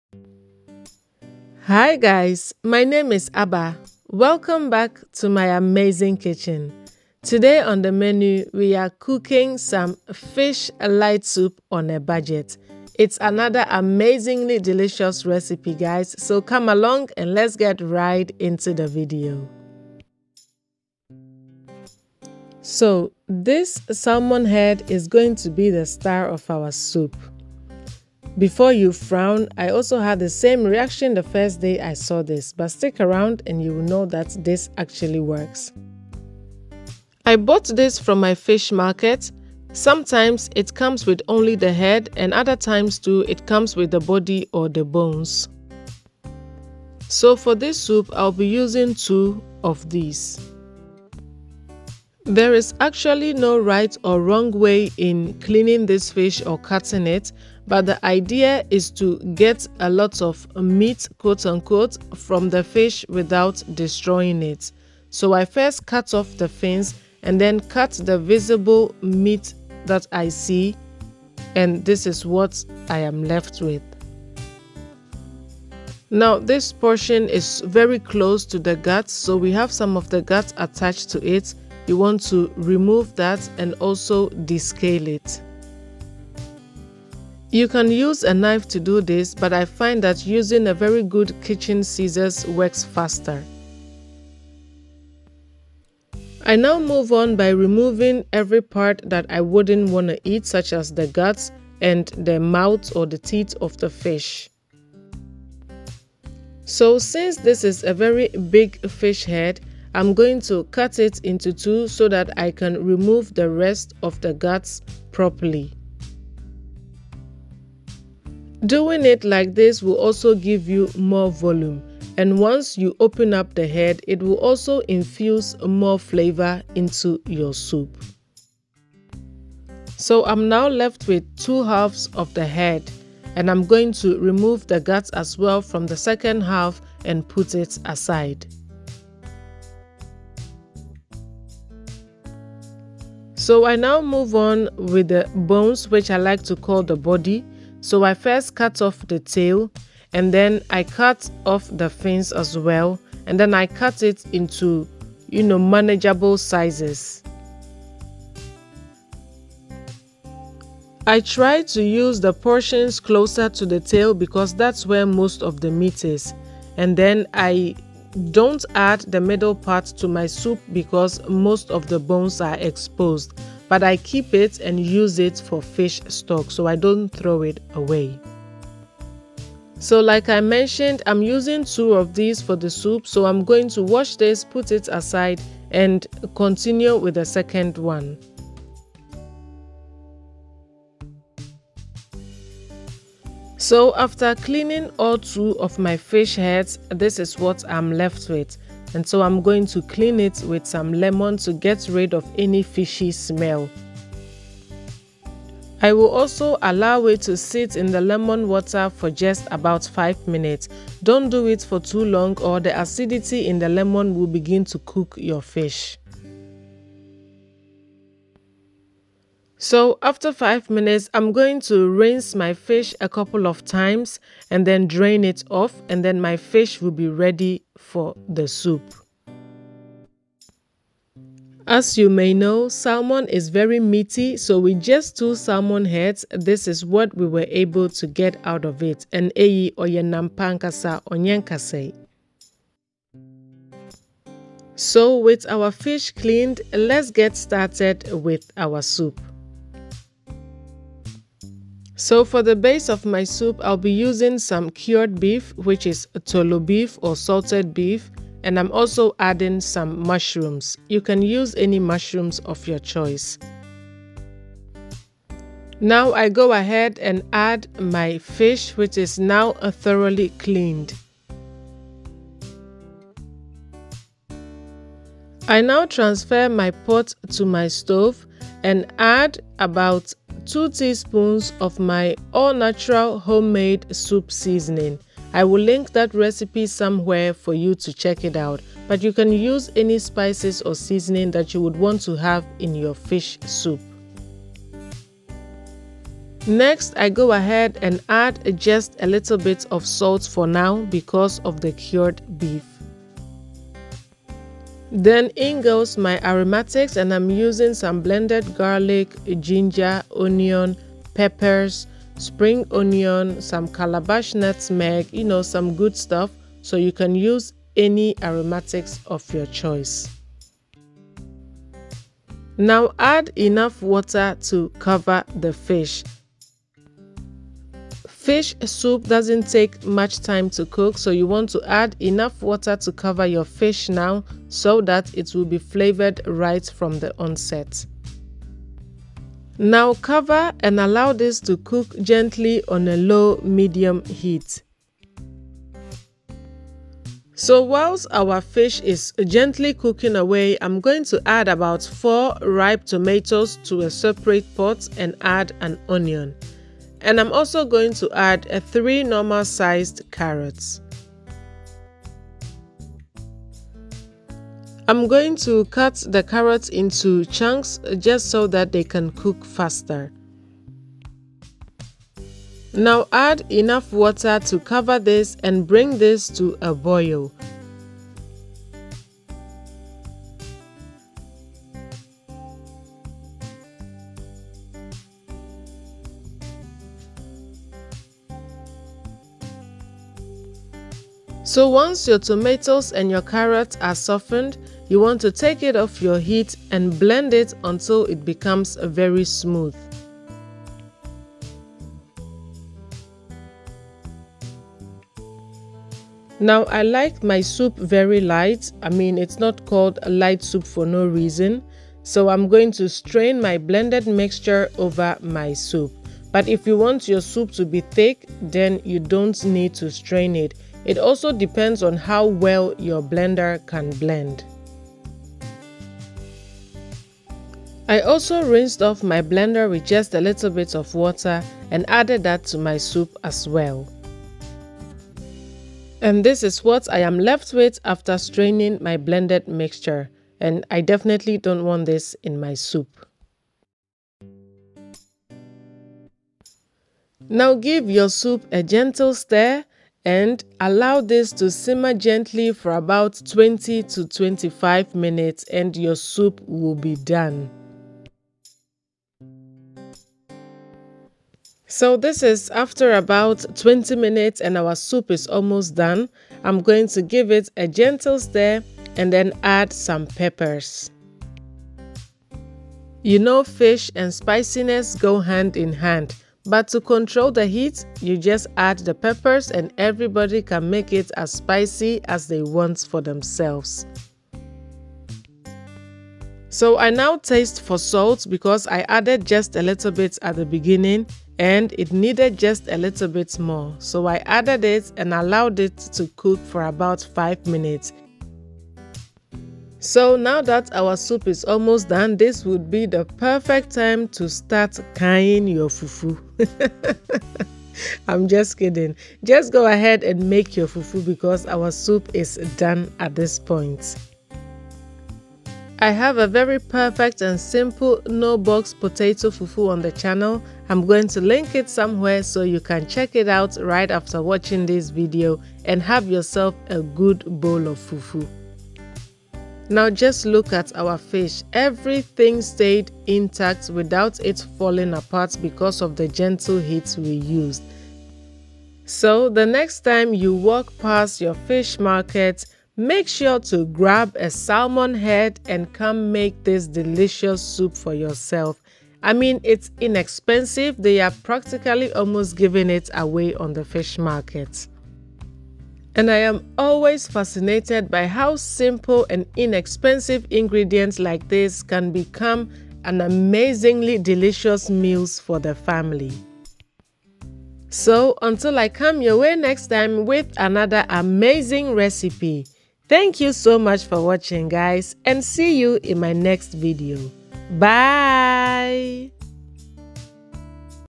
Fish Light Soup on a Budget || Salmon Soup || Amazin Kitchen
The first day I saw my aunty cook this soup, I frowned, because why will you use salmon head for soup? But I was pleasantly surprised when I tasted it. I hope you get the same experience when you try this recipe

Ingredients
2 salmon heads
50 gr cured beef
200 gr Mushrooms
2 teaspoons soup seasoning
3 tablespoons blended aromatics (ginger, garlic, pepper, onion, etc)

4 fresh ripe tomatoe
1 onion
3 carrots
About 3 liters of water